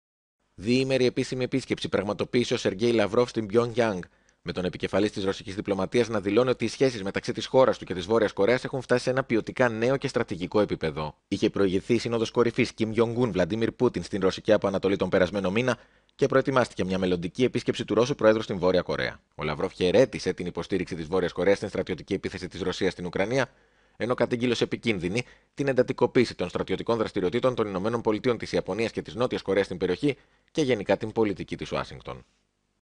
Στη Β. Κορέα ο Σ. Λαβρόφ: Ευχαριστίες στην Πιονγκγιάνγκ για την υποστήριξη στον πόλεμο της Ουκρανίας
Πλήρη υποστήριξη στη Βόρεια Κορέα υπόσχεται ο Ρώσος υπουργός Εξωτερκών που προετοιμάζει την ιστορική επίσκεψη Πούτιν